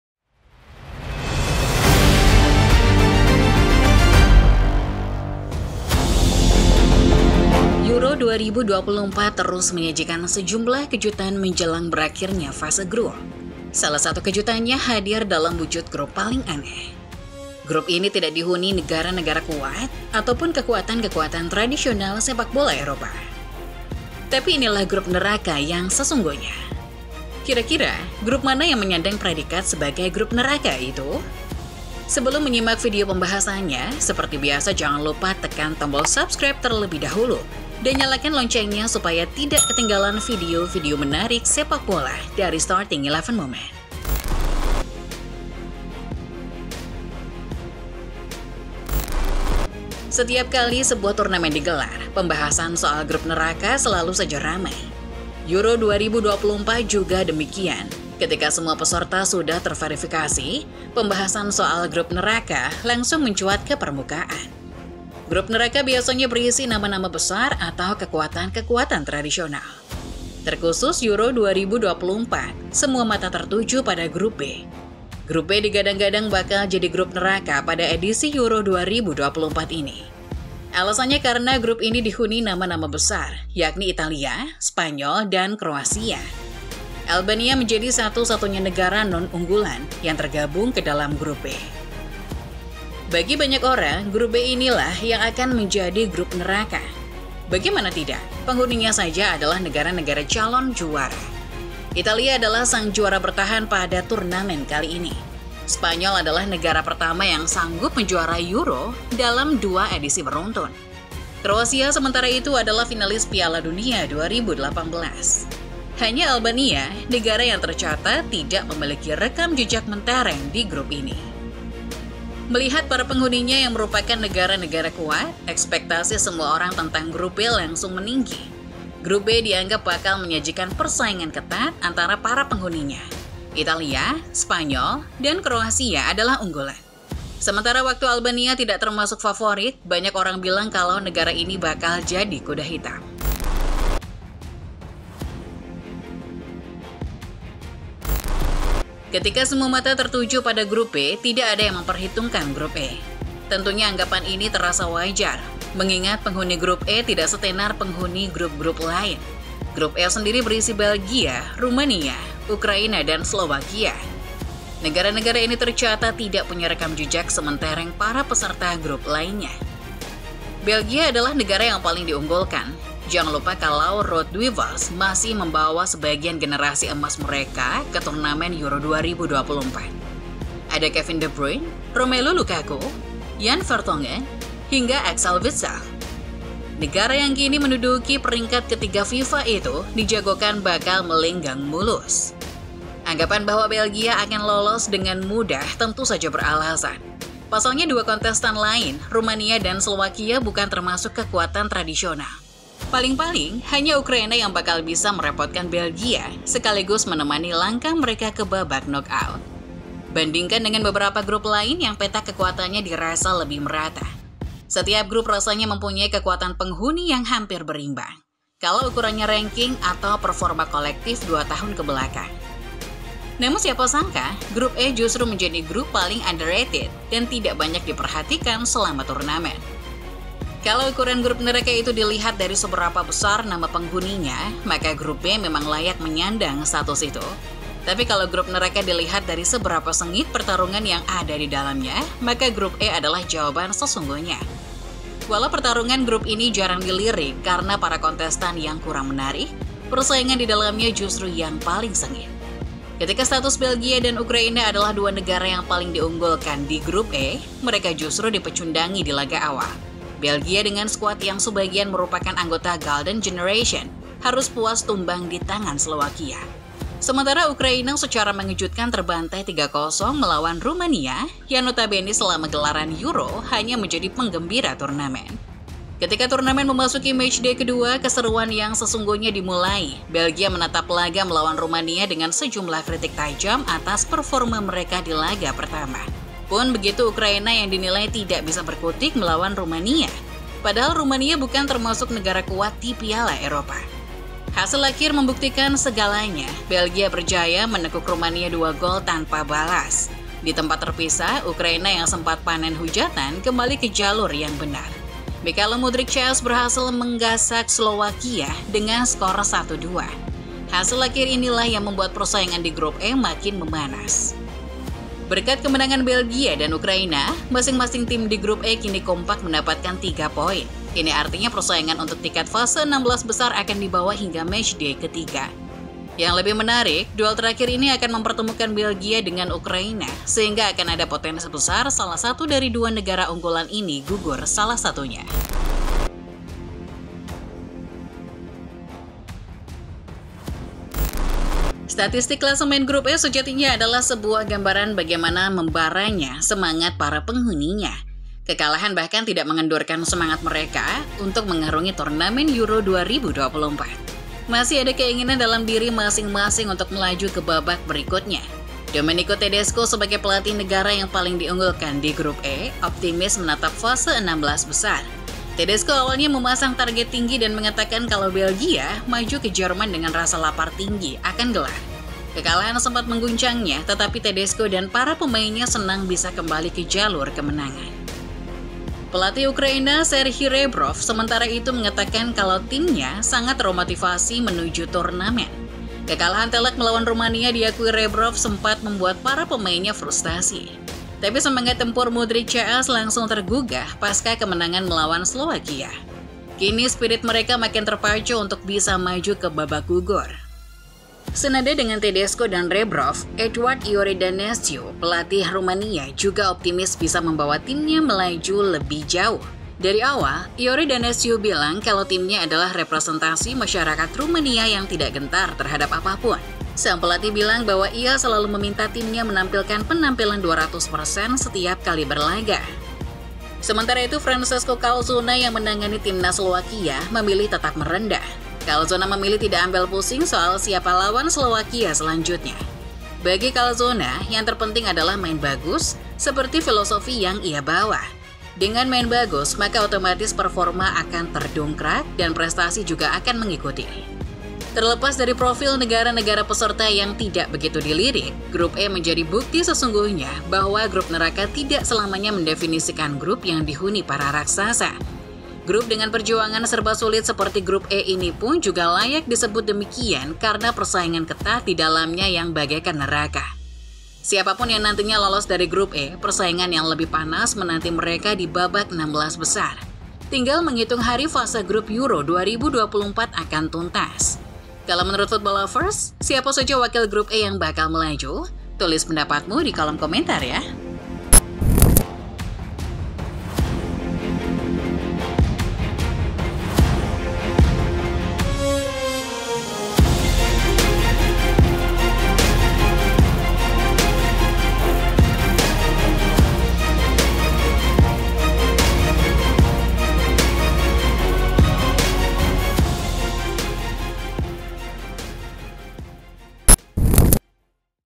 Grup Paling Aneh di Euro 2024! Underrated Tapi Jadi Grup Neraka yang Sesungguhnya
Euro 2024 terus menyajikan sejumlah kejutan menjelang berakhirnya fase grup. Salah satu kejutannya hadir dalam wujud grup paling aneh. Grup ini tidak dihuni negara-negara kuat ataupun kekuatan-kekuatan tradisional sepakbola Eropa. Tapi inilah grup neraka yang sesungguhnya! Kira-kira, grup mana yang menyandang predikat sebagai grup neraka itu?